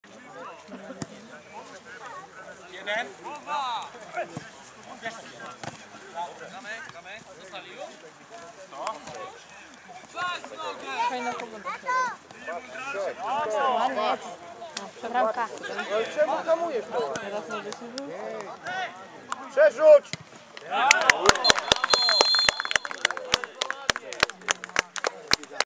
GLKS Jonkowo OKS 1945 II Stomilowcy Olsztyn piąta bramka meczu Krzysztof Kłos
bramka na 3:2 Krzysztof Kłos